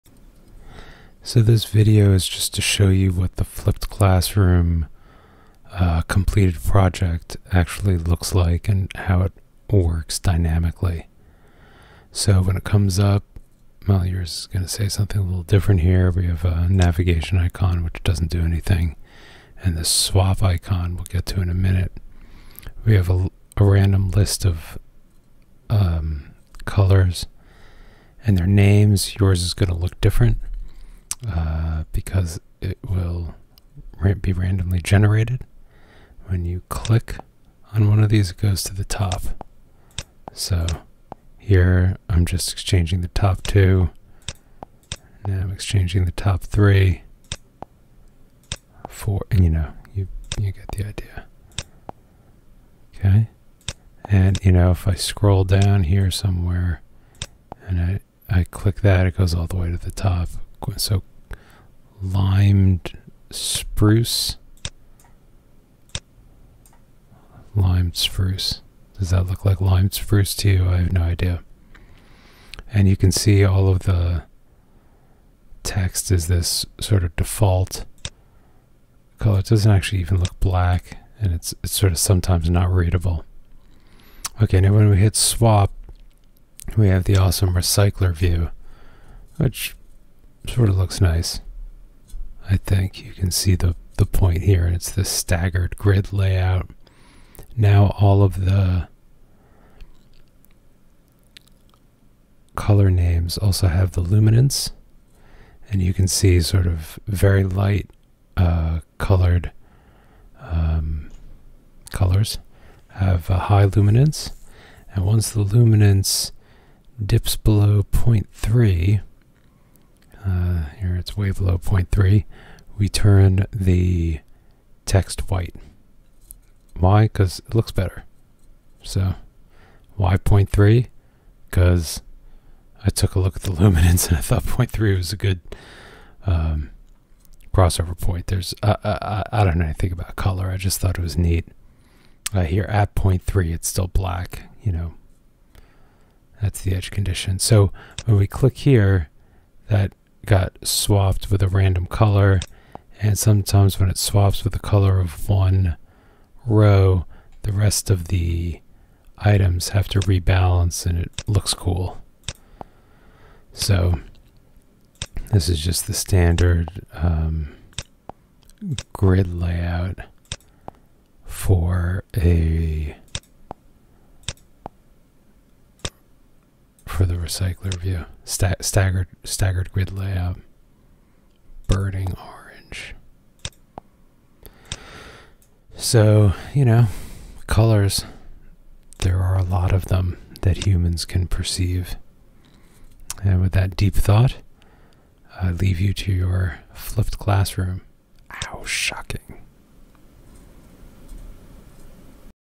ListView/RecyclerView flipped class demo
This is a demonstration of the completed code for the ListView/RecyclerView flipped classroom.